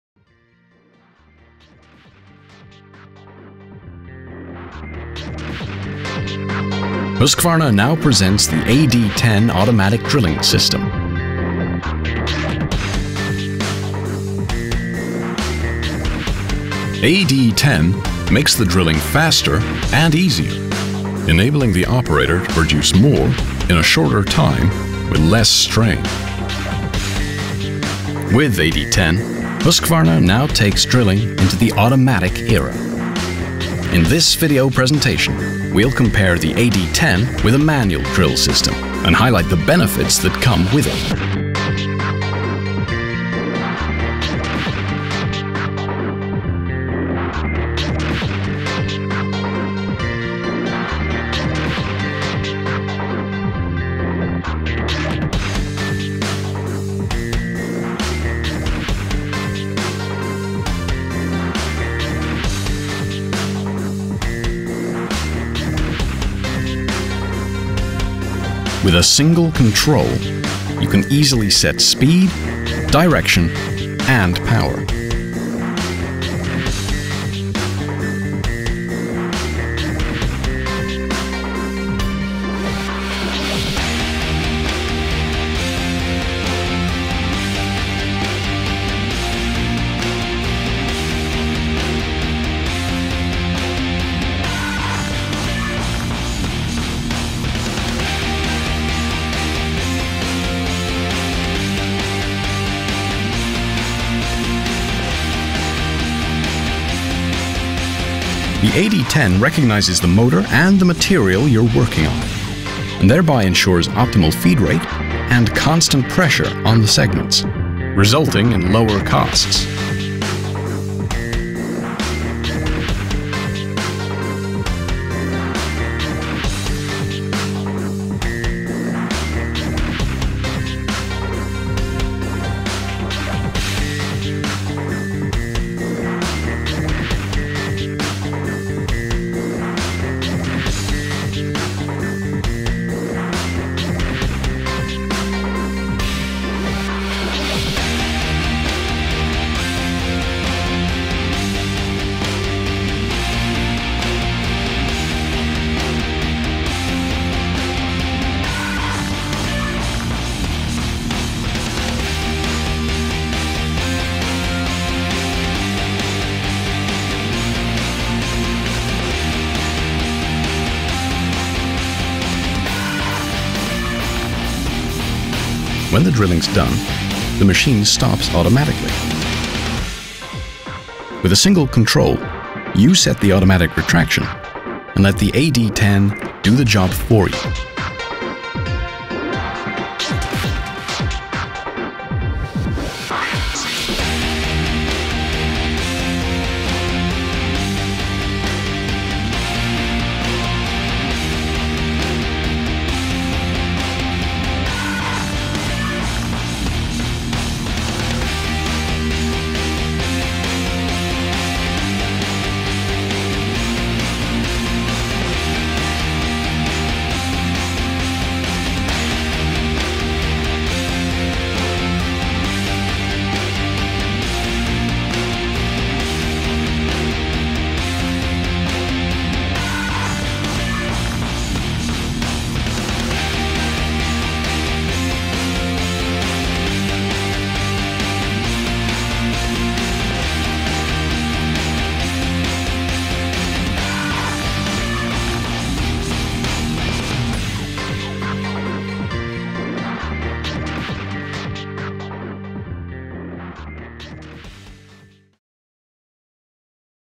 AD 10 vs manual drilling
Automatic drilling versus manual drilling.

In this video we compare an automatic drilling system with a manual drilling system.
Both systems consist of drillmotor Husqvarna DM 340 and drill stand Husqvarna DS 450. The difference is that Husqvarna automatic drill unit, AD 10 is included in the automatic drill system.

The video shows the advantages with AD 10:
Cost-effective; You can produce more holes within the same time. AD 10 secure a constant pressure of the drilling which means less wear on the segments which means increased tool life.
Easy; Easy to use and easy to install. AD 10 senses which drill motor is being used and automatically optimises the feed rate. When the hole is done AD 10 stops automatically and the user can set the regulator of the AD 10 to automatically retract the drill core.
Ergonomic: You can stand comfortably upright and thereby reduce the body stress.
With the aid of one control the user controls speed, direction and power.

Speed 0-3,2m/min in both directions
AD 10 weighs only 3,6 kg (7,9 lbs)